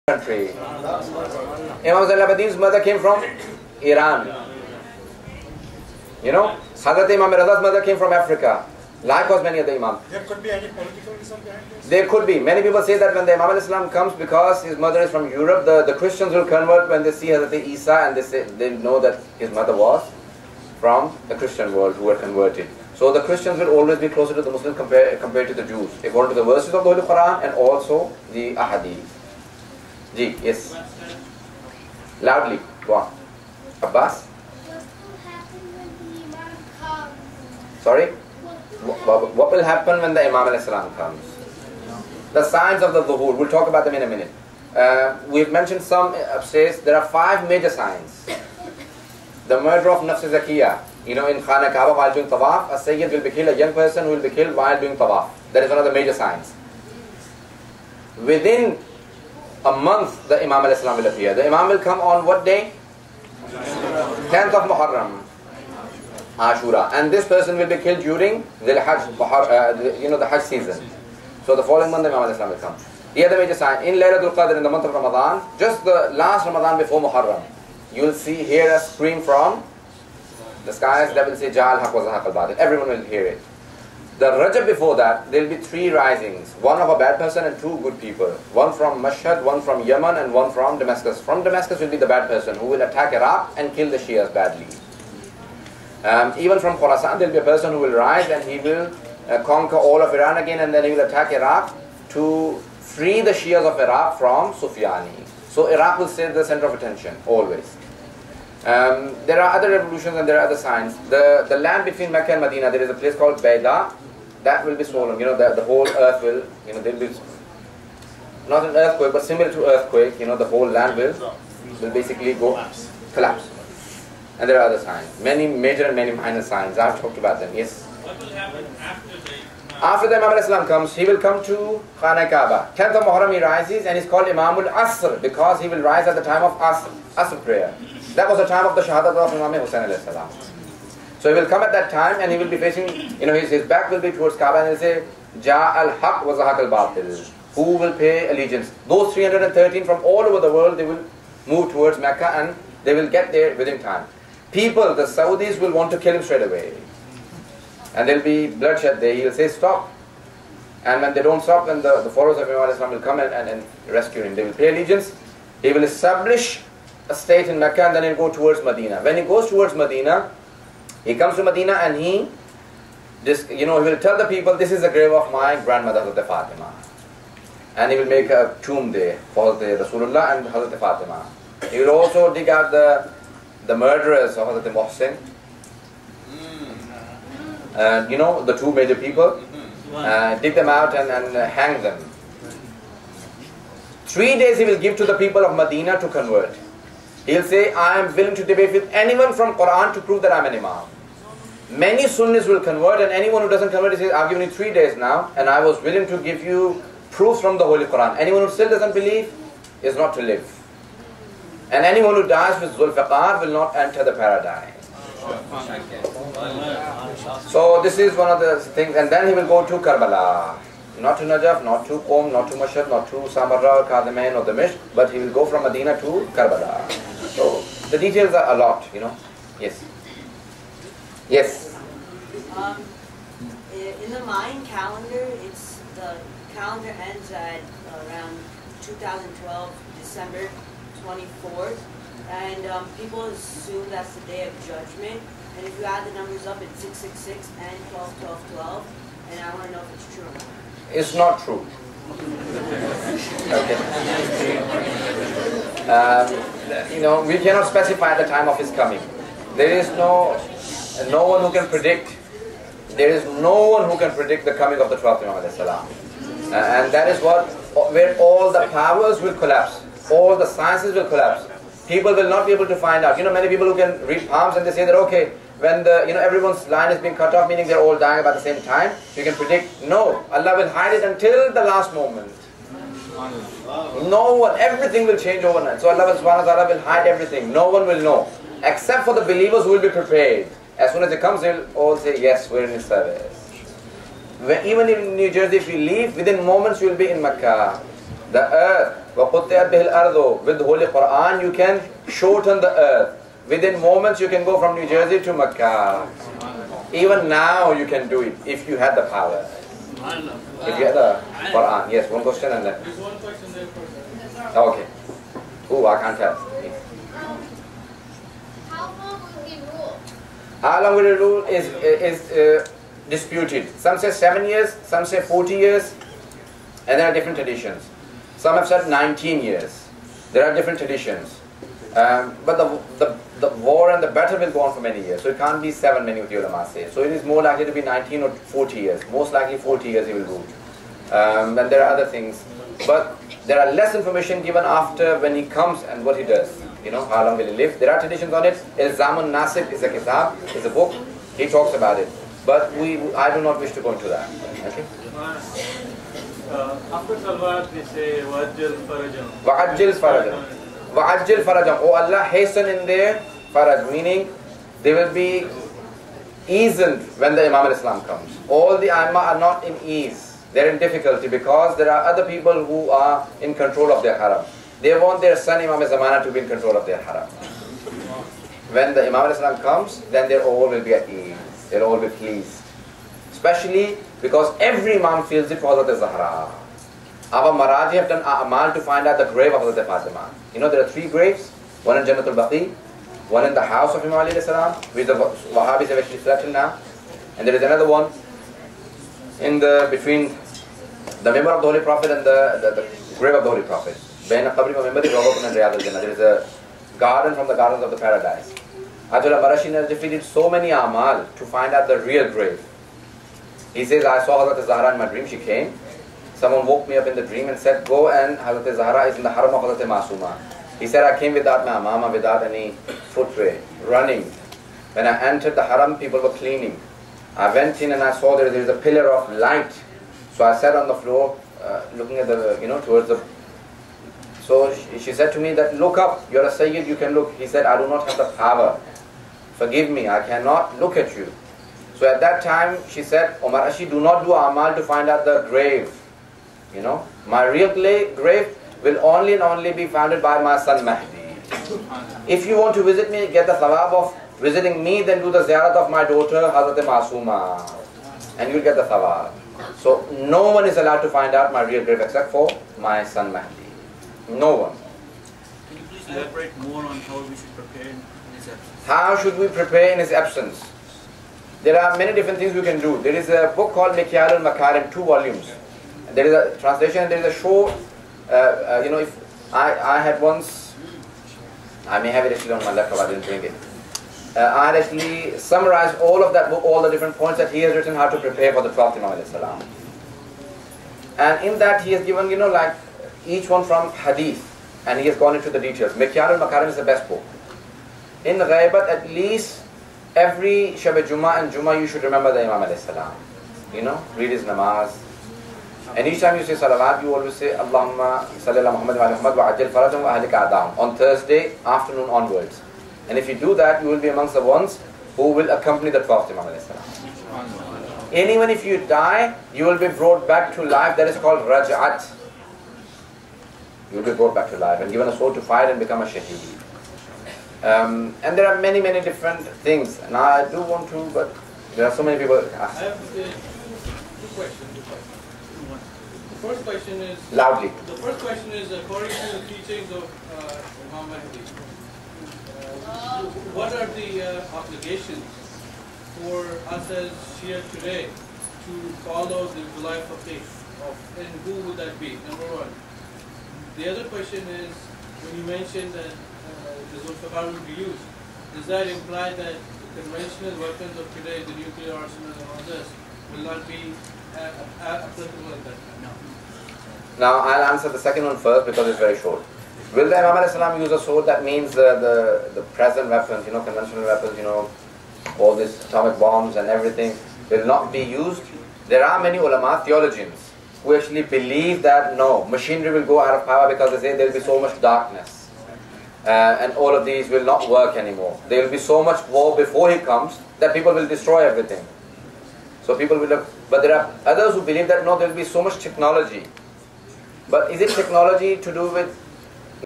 Quran and Hadith Lecture Series - Imam Mehdi (atfs) (Part 5)
Quran and Hadith Lecture Series with Moulana Ali Raza Rizvi held at Astaana-e-Zehra during Ramadan 2008